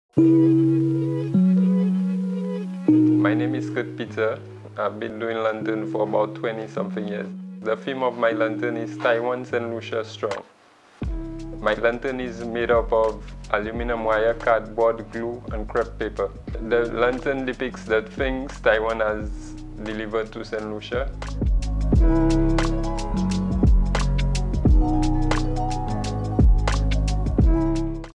The Second Annual Lantern Competition presents - Kurt Peter
Held under the theme "Taiwan Saint Lucia Strong," the annual National Lantern Competition is held in commemoration of the Republic of China (Taiwan) National Day Celebrations.
A message from the Cultural Development Foundation of Saint Lucia.